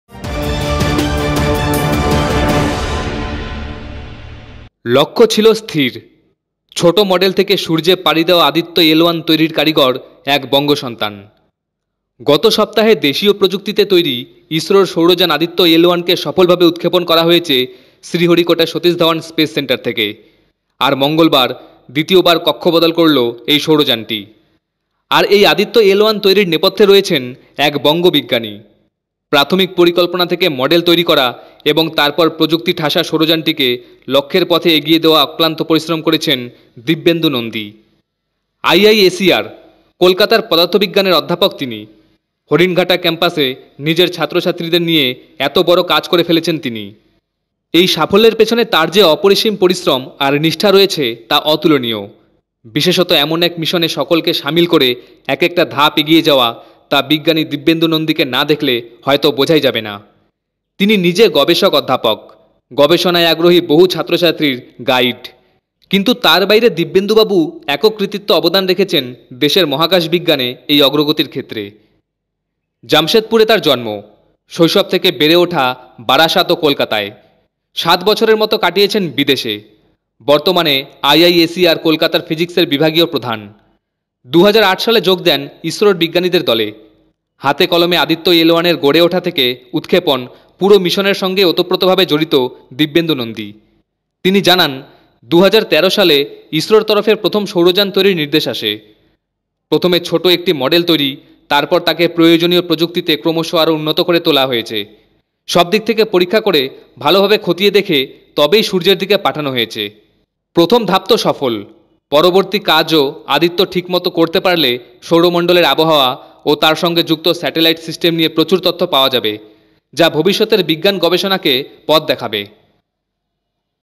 Aditya L-1: দেশীয় প্রযুক্তিতে তৈরি আদিত্য এল-১ এরও নেপথ্যে রয়েছে বাংলার এক বিজ্ঞানী | NEW2NEWZ
লক্ষ্য ছিল স্থির, ছোট মডেল থেকে সূর্যে পাড়ি দেওয়া ‘আদিত্য’ তৈরির কারিগর এক বঙ্গসন্তান। গত সপ্তাহে দেশীয় প্রযুক্তিতে তৈরি ইসরোর সৌরযান ‘আদিত্য এল১’কে  সফলভাবে উৎক্ষেপণ করা হয়েছে শ্রীহরিকোটা থেকে। আর মঙ্গলবার কক্ষপথ দ্বিতীয়বার বদল করল সৌরযানটি। আর এই ‘আদিত্য’ তৈরির নেপথ্যে রয়েছেন এক বঙ্গ বিজ্ঞানী। প্রাথমিক পরিকল্পনা থেকে মডেল তৈরি এবং তারপর প্রযুক্তি-ঠাসা সৌরযানটিকে লক্ষ্যের পথে এগিয়ে দেওয়ার অক্লান্ত পরিশ্রম করেছেন দিব্যেন্দু নন্দী। IISER, কলকাতার পদার্থবিজ্ঞানের অধ্যাপক তিনি। হরিণঘাটা ক্যাম্পাসে নিজের ছাত্রছাত্রীদের নিয়েও এত বড় কাজ করে ফেলেছেন তিনি।

এই সাফল্যের পিছনে তার যে অপরিসীম পরিশ্রম আর নিষ্ঠা রয়েছে, তা অতুলনীয়। বিশেষত এমন এক মিশনে সকলকে শামিল করে একেকটা ধাপ এগিয়ে যাওয়া, তা বিজ্ঞানী দিব্যেন্দু নন্দীকে না দেখলে হয়ত বোঝাই যাবে না। তিনি নিজে গবেষক-অধ্যাপক। গবেষণায় আগ্রহী বহু ছাত্রছাত্রীর ‘গাইড’। কিন্তু তার বাইরে দিব্যেন্দুবাবু একক কৃতিত্বে অবদান রেখেছেন দেশের মহাকাশ বিজ্ঞানে এই অগ্রগতির ক্ষেত্রে।

জামশেদপুরে তাঁর জন্ম। শৈশব থেকে বেড়ে ওঠা বারাসত ও কলকাতায়। ৭ বছরের মত কাটিয়েছেন বিদেশে।  বর্তমানে IISER, কলকাতার ফিজিক্সের বিভাগীয় প্রধান। ২০০৮ সালে যোগ দেন ইসরোর বিজ্ঞানীদের দলে। হাতেকলমে ‘আদিত্য এল১’-এর গড়ে ওঠা থেকে উৎক্ষেপণ – পুরো মিশনের সঙ্গেই ওতোপ্রোতোভাবে জড়িত দিব্যেন্দু নন্দী। তিনি জানান, ২০১৩ সালে ইসরোর তরফে প্রথম সৌরযান তৈরির নির্দেশ আসে। প্রথমে ছোট একটি মডেল তৈরি করে তারপর তাকে প্রয়োজনীয় প্রযুক্তিতে ক্রমশ আরও উন্নত করে তোলা হয়েছে। সব দিক পরীক্ষা করে ভালভাবে খতিয়ে দেখে তবেই সূর্যের দিকে পাঠানো হয়েছে। প্রাথমিক ধাপ তো সফল। পরবর্তী কাজও ‘আদিত্য’ ঠিকমতো করতে পারলে সৌরমণ্ডলের আবহাওয়া ও তার সঙ্গে যুক্ত স্যাটেলাইট সিস্টেম নিয়ে প্রচুর তথ্য পাওয়া যাবে, যা ভবিষ্যতের বিজ্ঞান গবেষণাকে পথ দেখাবে।